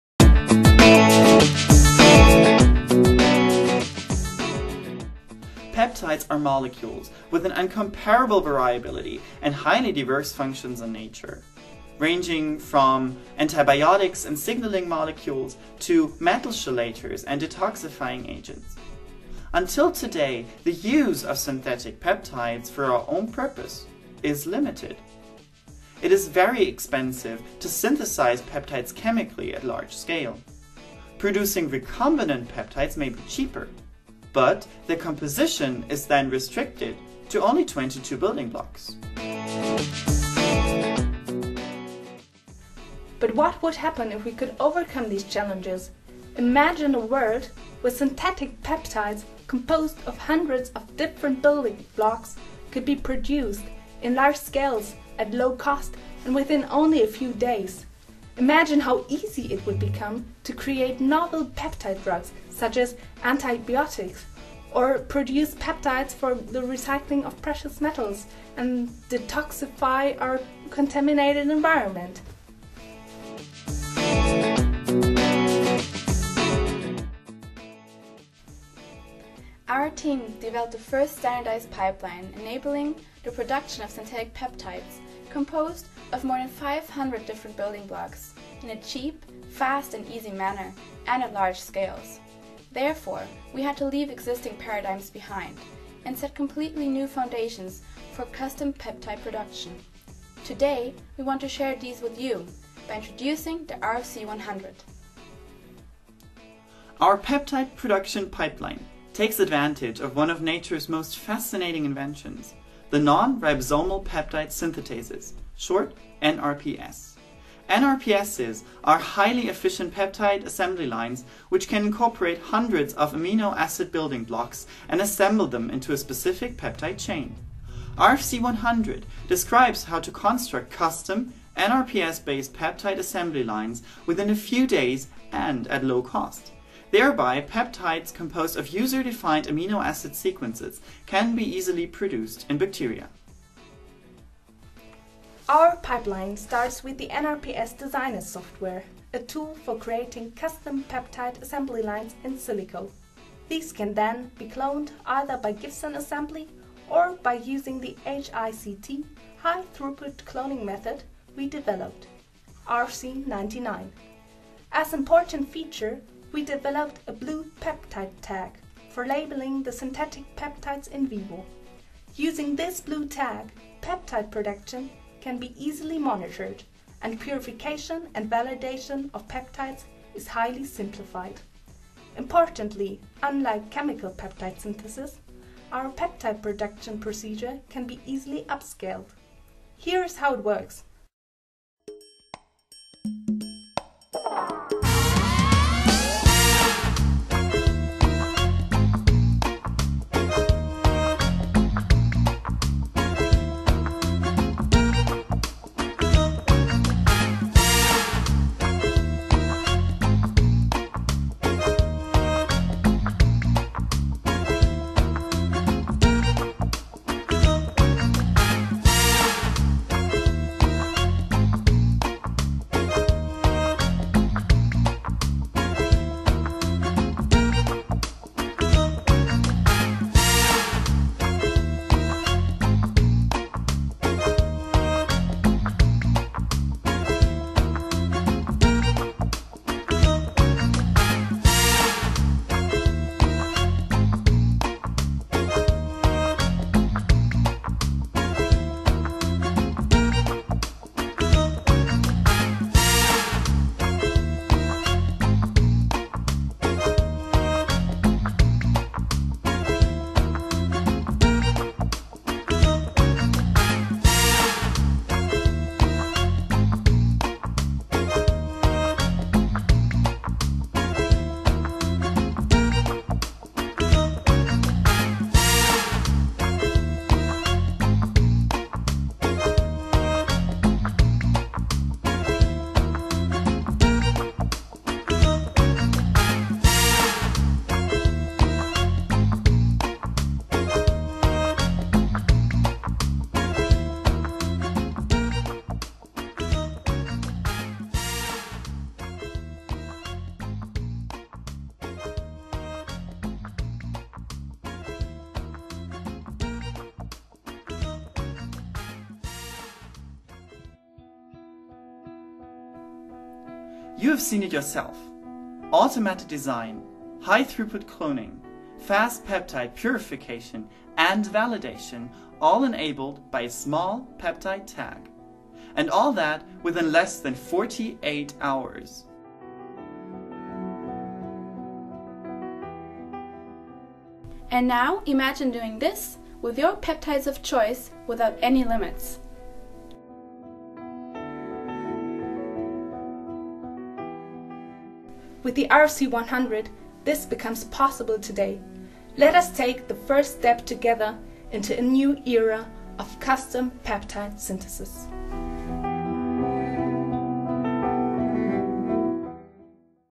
RFC100 Trailer - iGEM Heidelberg 2013
This is an introduction video on our pipeline for the design and production of custom non-ribosomal peptides from the first idea up to industrial production. Non-Ribosomal Peptides can have a myriad of functions.